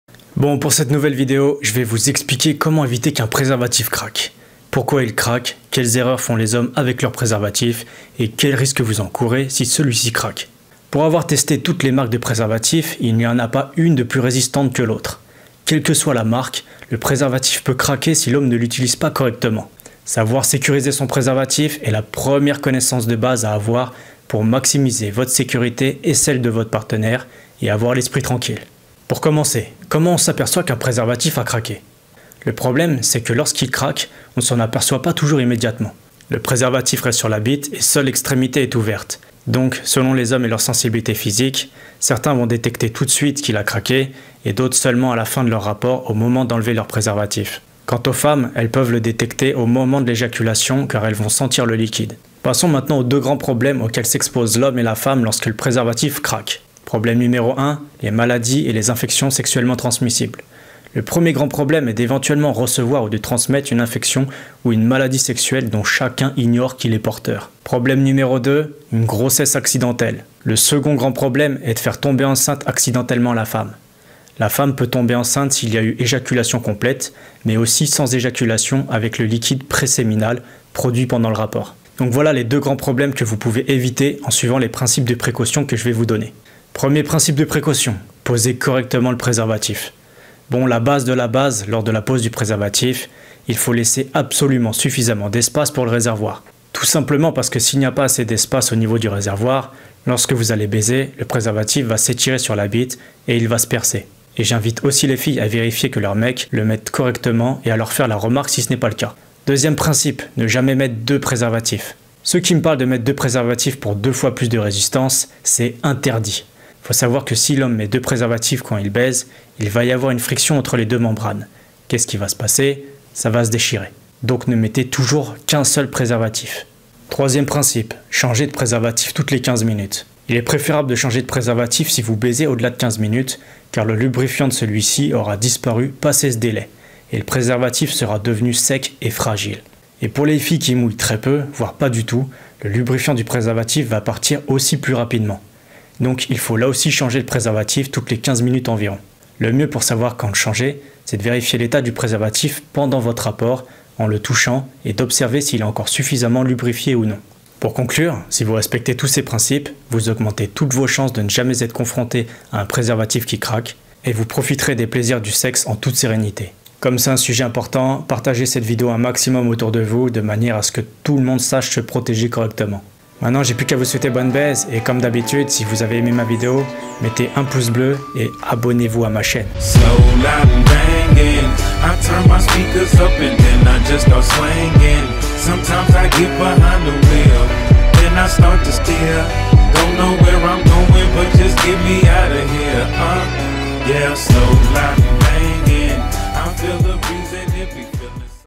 Comment éviter qu'un préservatif craque
Découvrez pourquoi un préservatif peut craquer et quelles sont les bonnes précautions à avoir pour éviter ce genre de problème.

SOMMAIRE
00:00 - Introduction
00:28 - Comment on s'aperçoit qu'un préservatif a craqué
00:52 - Les 2 problèmes lorsque le préservatif craque
01:26 - 1er principe de précaution : poser correctement le préservatif
01:49 - 2ème principe de précaution : ne jamais mettre 2 préservatifs
02:07 - 3ème principe de précaution : changer de préservatif toutes les 15 min
02:37 - Conclusion